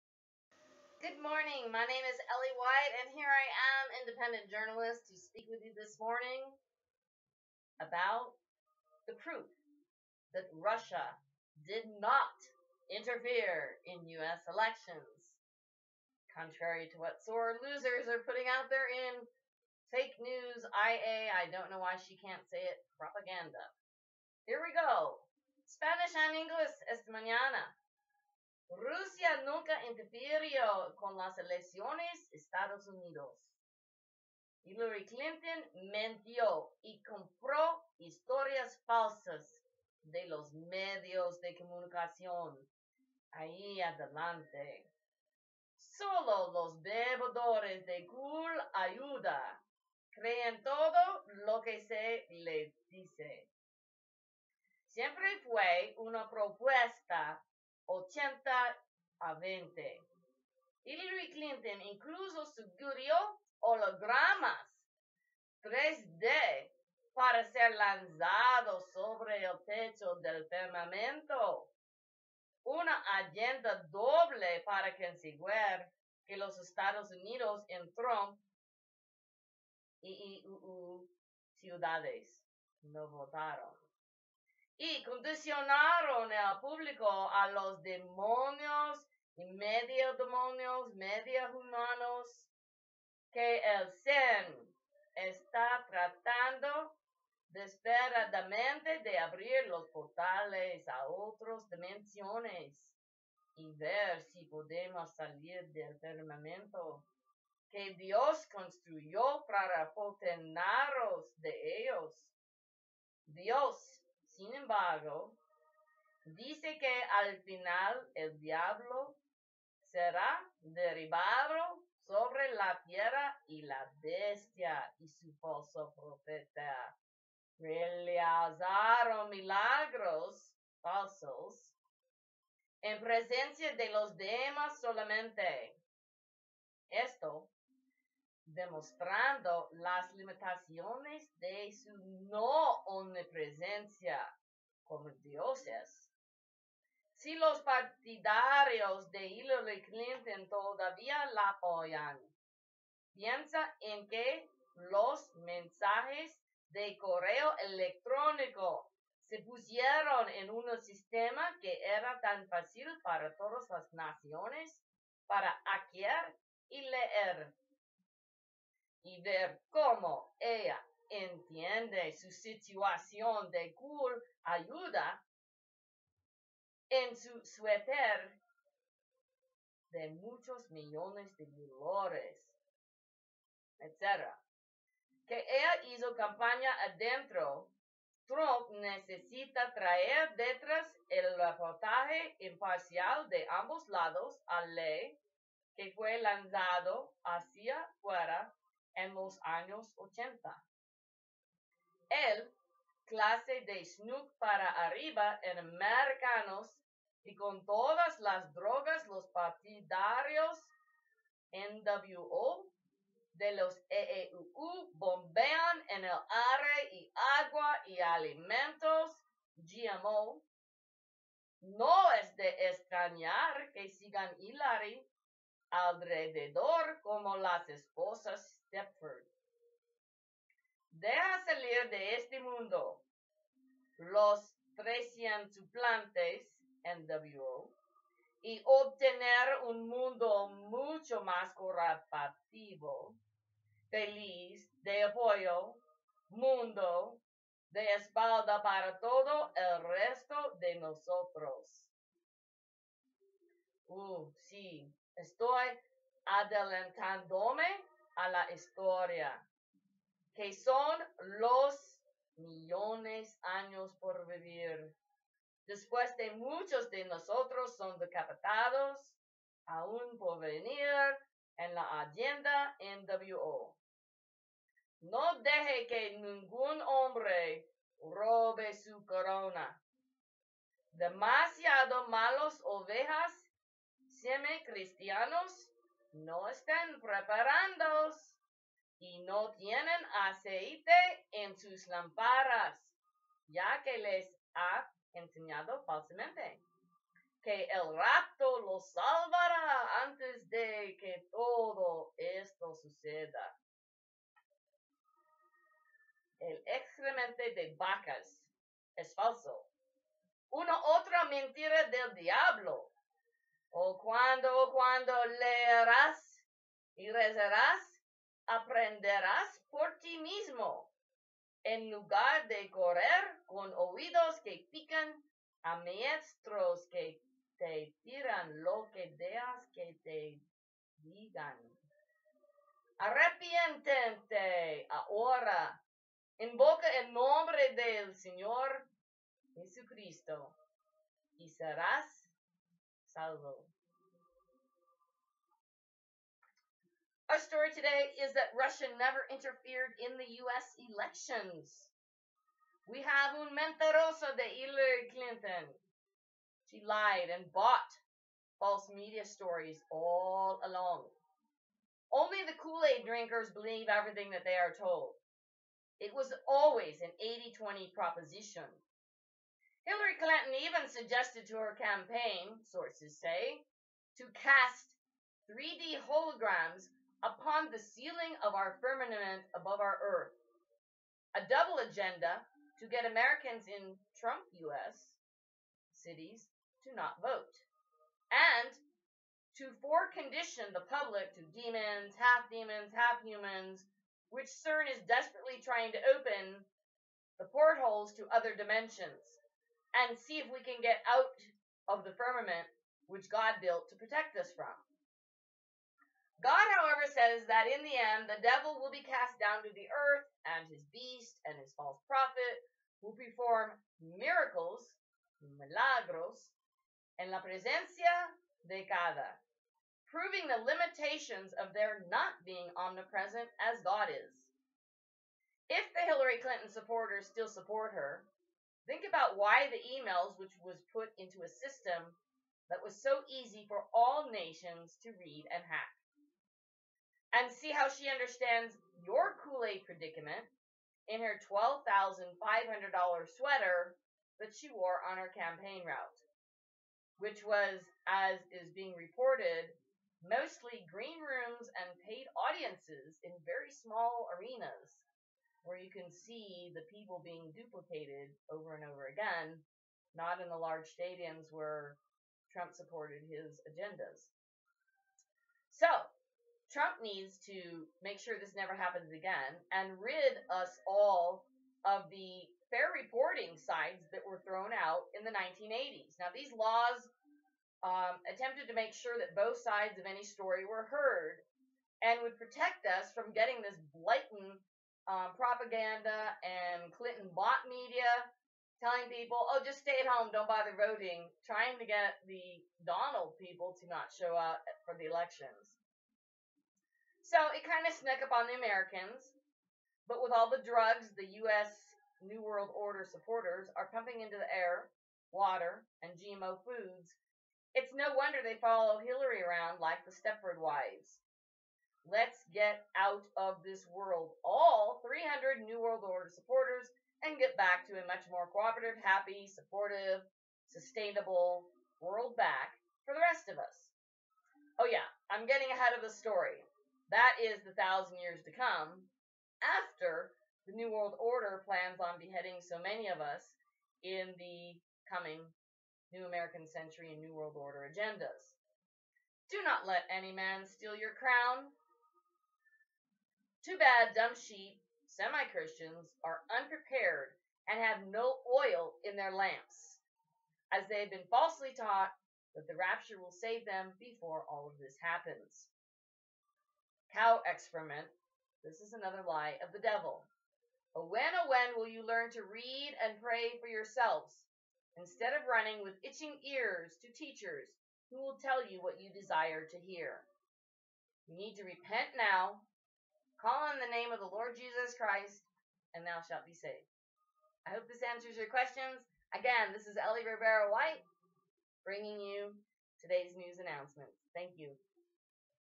Ellie White PROOF RUSSIA Interference in US Elections is FALSE
Hillary created a Fake World War III to hide her plans to destroy and destabilize any friendly to Russia...so...HER Experience in Politics would win over TRUMP... but when Trump met with real masses and HC did everything Green Screen. then.